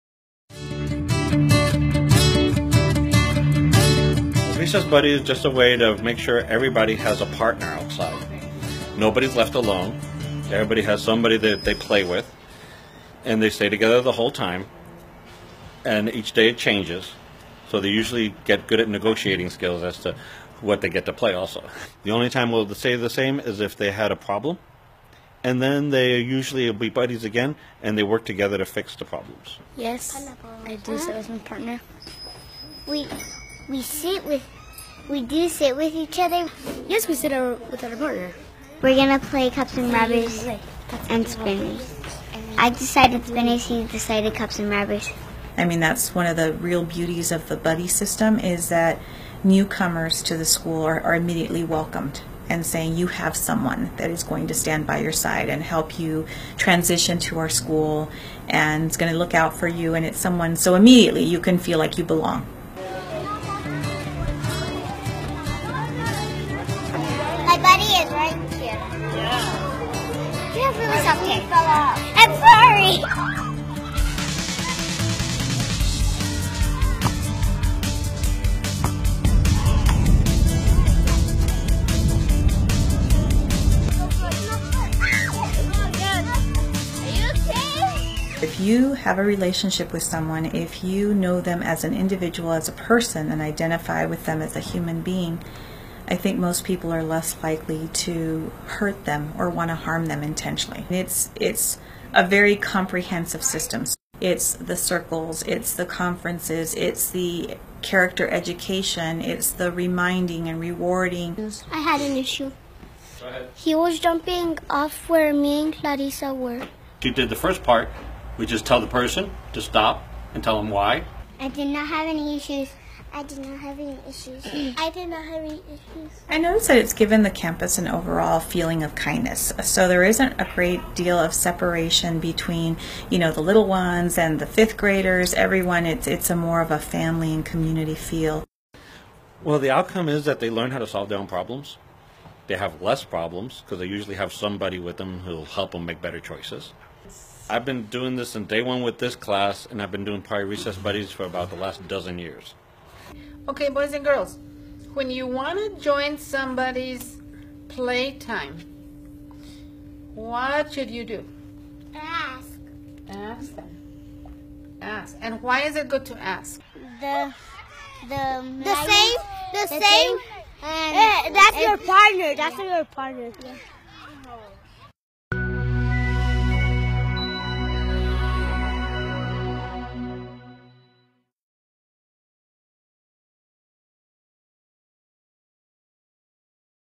TUSD1 - Recess Buddies Rock This School!
Mission View Elementary School has a remarkable buddy system that has helped create an inclusive and supportive campus.